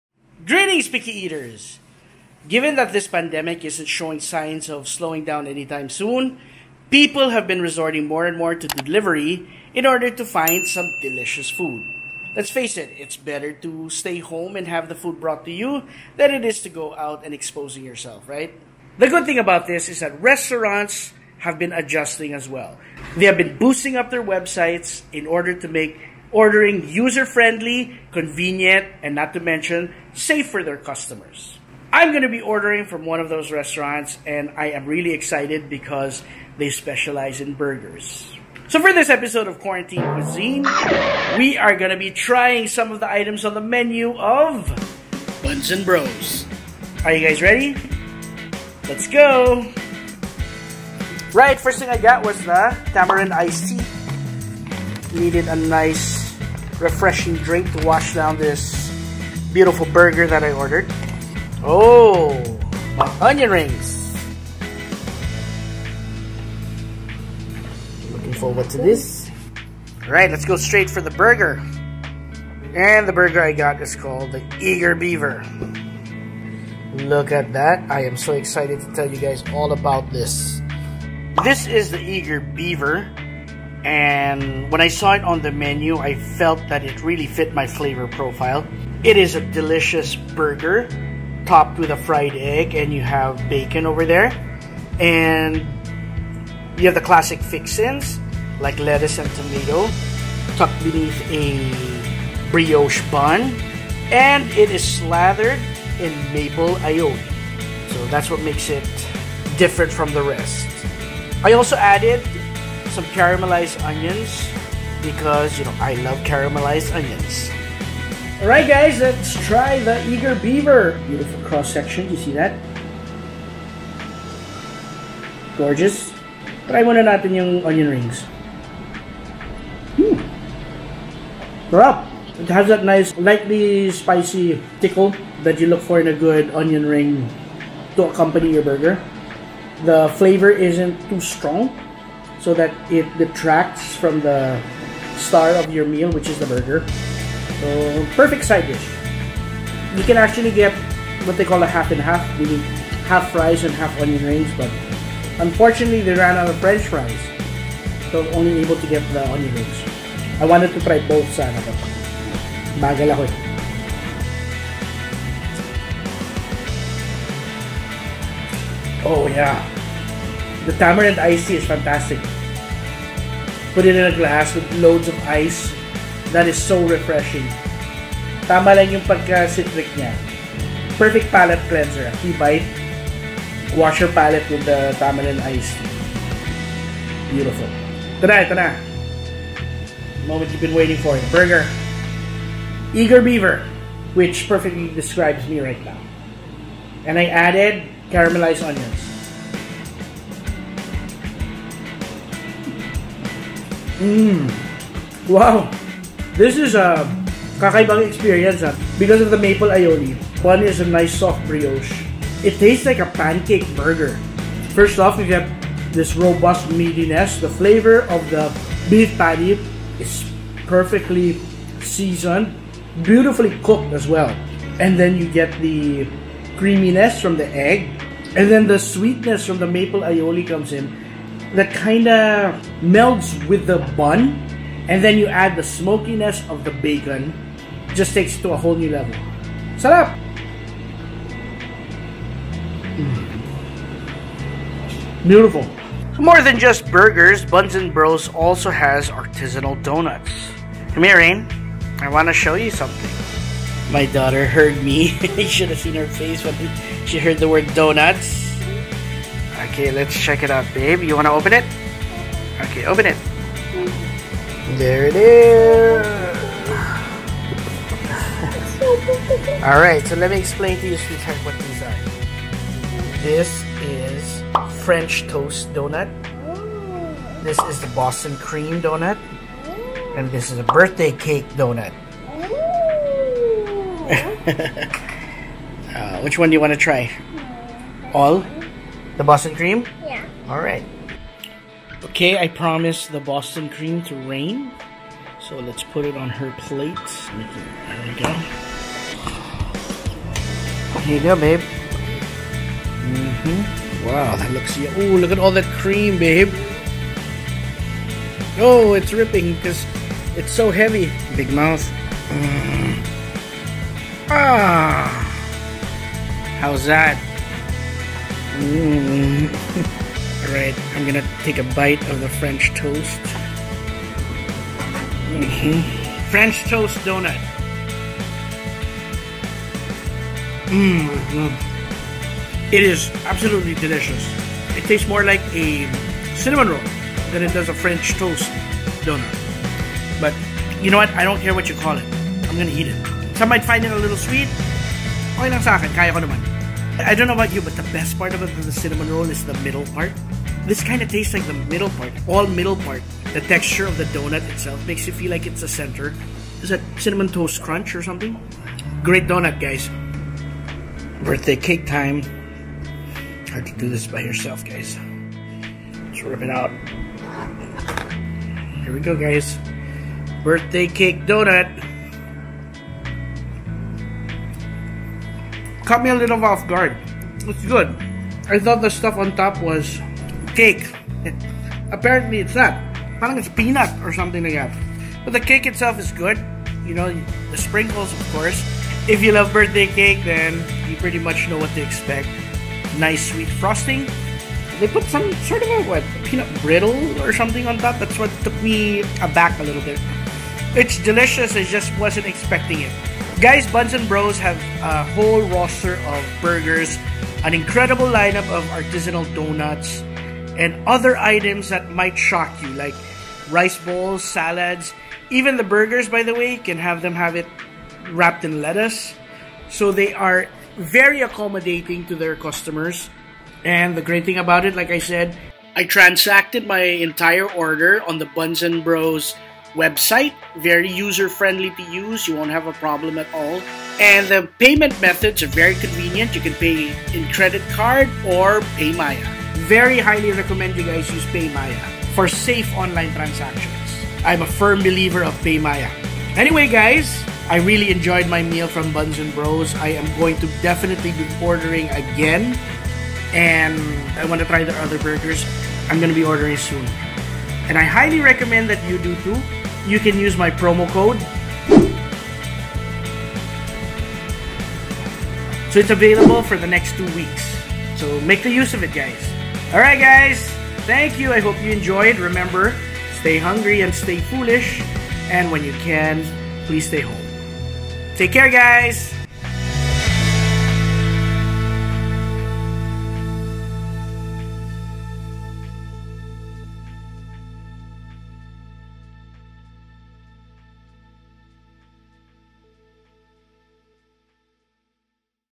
QUARANTINE CUISINE #32: DELIVERY VIA THE BUNS & BRO WEBSITE!
Even before the Pandemic began, Buns & Bro had established itself as a rising Burger brand.

They even opened their first full brick and mortar branch located at Ayala North Exchange in Makati.

Because of the current situation, Buns & Bro had to adapt to the new normal, and they did so masterfully by upgrading their website to make ordering delivery incredibly easy and hassle free!

BNBPICKIESTEATER20